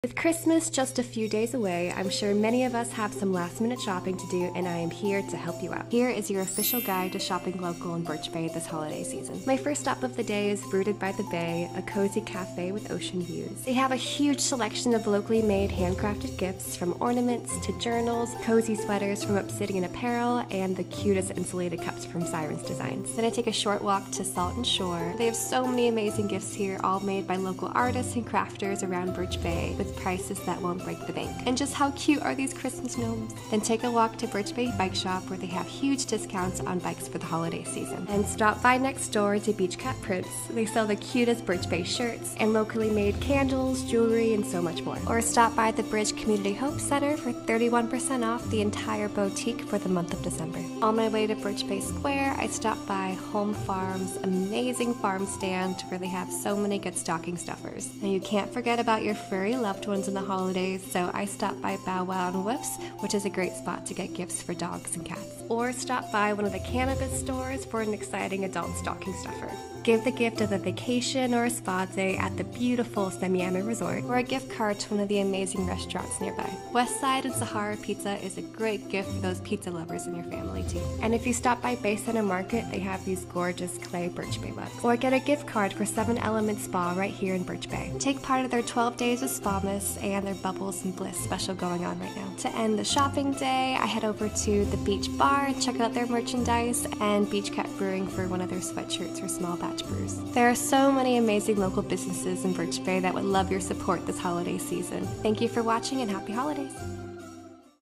Birch Bay at Christmas!
There are so many amazing businesses in and around Birch Bay that are offering beautiful presents at great prices! Find the perfect gift for someone you love while shopping local! 🎁🎄🎅